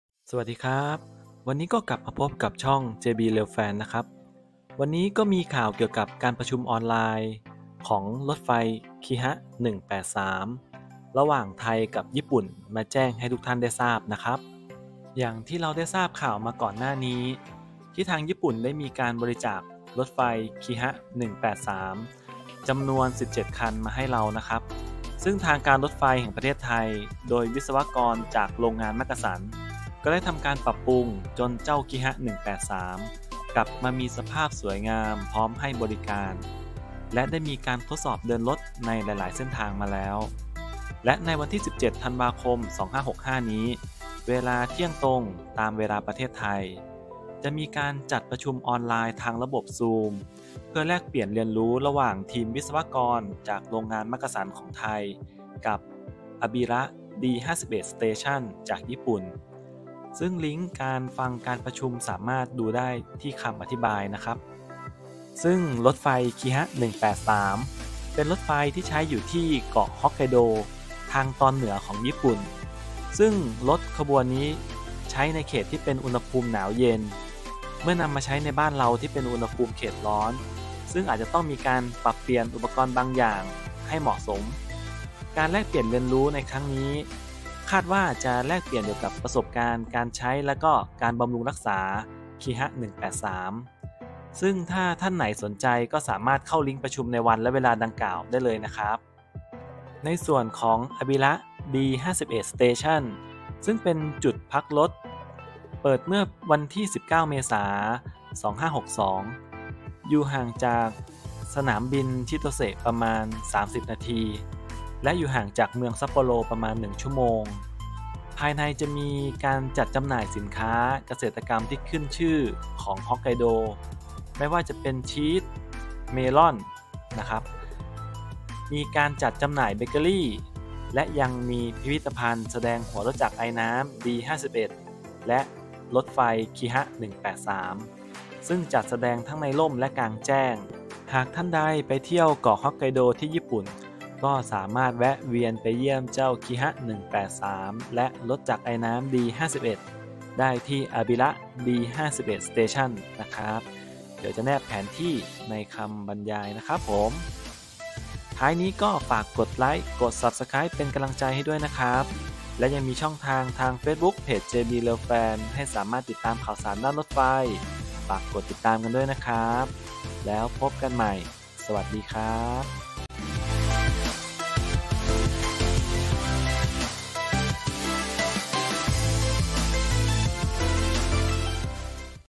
Kiha 183 ไทย+ญี่ปุ่น จัดประชุมออนไลน์ร่วมกัน
ในวันที่ 17 ธันวาคม 2565 เวลา 12.00 น. ตามเวลาประเทศไทย จะมีการจัดประชุมออนไลน์ทางระบบ zoom เพื่อแลกเปลี่ยนเรียนรู้ระหว่างทีมวิศวกรจากโรงงานมักกะสันของไทยกับ Abira D51 station จากญี่ปุ่น ซึ่ง Link เข้าฟังการประชุมสามารถดูได้ที่คำอธิบายนะครับ ซึ่งรถไฟ Kiha 183 เป็นรถไฟที่ใช้อยู่ที่เกาะฮอกไกโดทางตอนเหนือของญี่ปุ่น ซึ่งรถขบวนนี้ใช้ในเขตที่เป็นอุณหภูมิหนาวเย็น เมื่อนำมาใช้ในบ้านเราที่เป็นอุณหภูมิเขตร้อนซึ่งอาจจะต้องมีการปรับเปลี่ยนอุปกรณ์บางอย่างให้เหมาะสม การประชุมแลกเปลี่ยนเรียนรู้ในครั้งนี้ คาดว่าจะแลกเปลี่ยนเกี่ยวกับประสบการณ์การใช้และการบำรุงรักษา Kiha 183 ซึ่งถ้าท่านไหนสนใจก็สามารถเข้าลิงค์ประชุมในวันและเวลาดังกล่าวได้เลยนะครับ

ที่มาของภาพและเนื้อหา